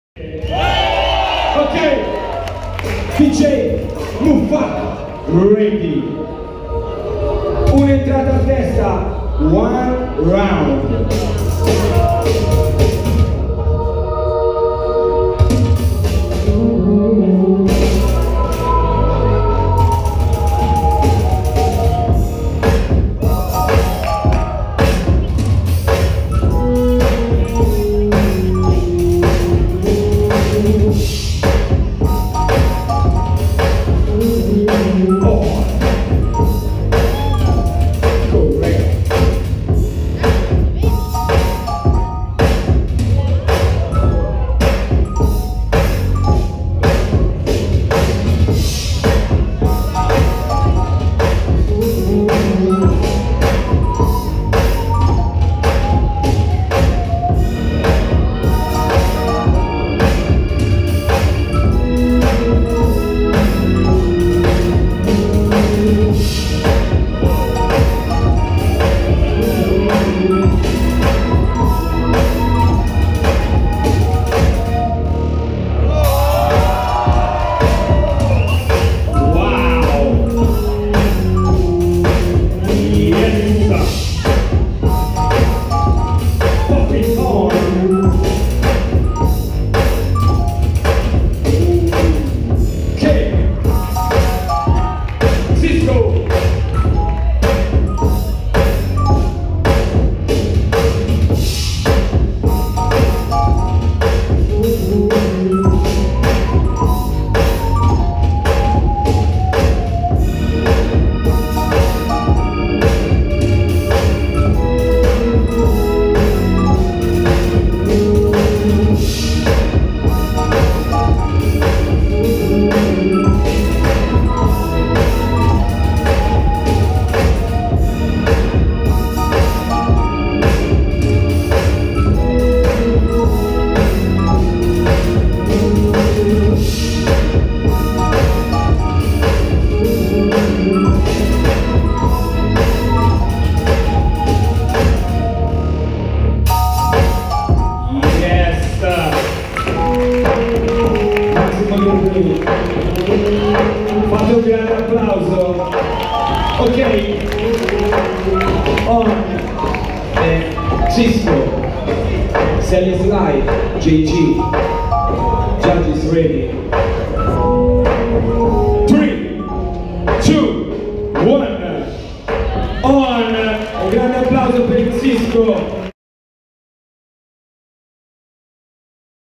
BACK TO THE STYLE POPPIN'' – 1st Round - HOAN vs XISCO
Back To the Style (BTS) 2K16 Pisa.
Ottavi di finale sezione poppin' Hoan vs Xisco winner Hoan
DJ: Mofak